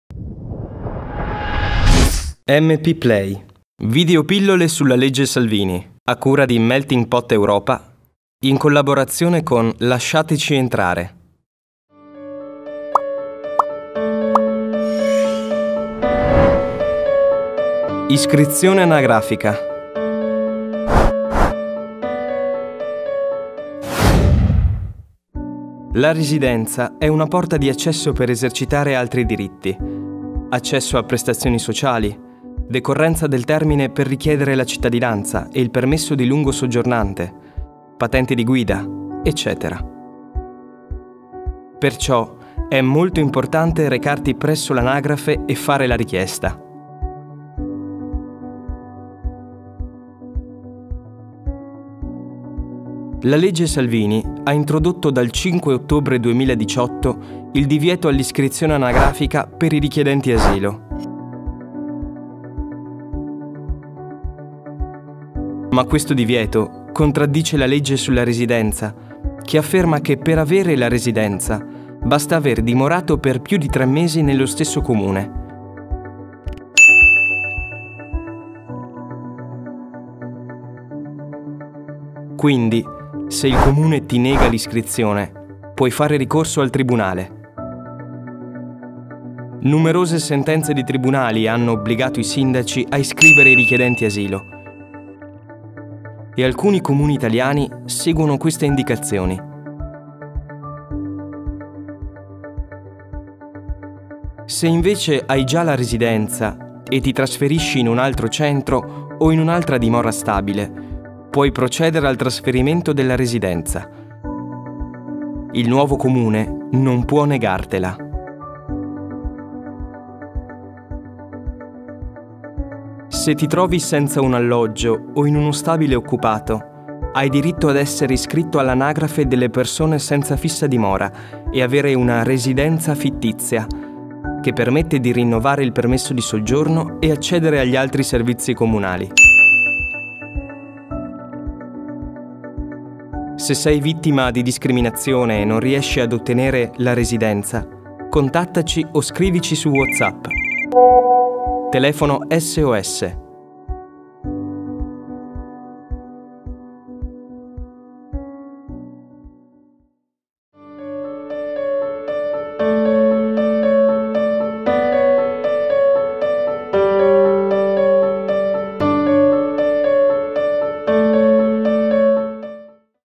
MP PLAY - تسجیل(الخانة) البیانات الشخصیة في البلدیة
فيديو إرشادي قصير حول قانون سالفيني